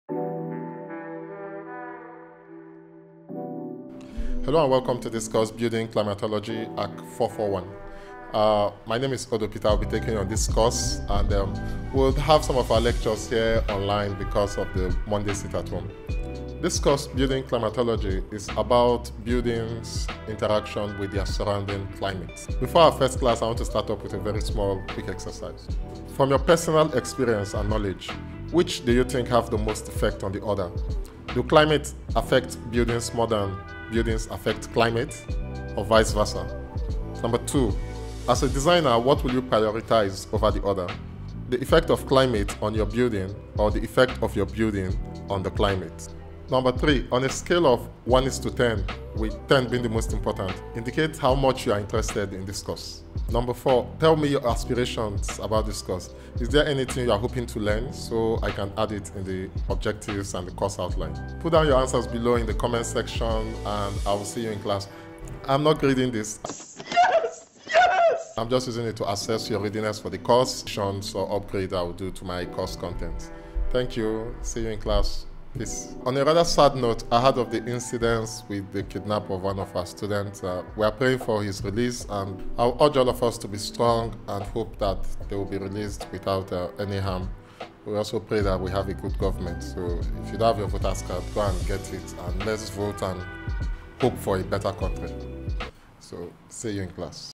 FIRST THOUGHTS ON BUILDINGS AND THE CLIMATE.
I lecture the course Building Climatology @ the department of Architecture, University of Nigeria Nsukka (located at the Enugu Campus.).

This Video series will contain extracts from the lectures.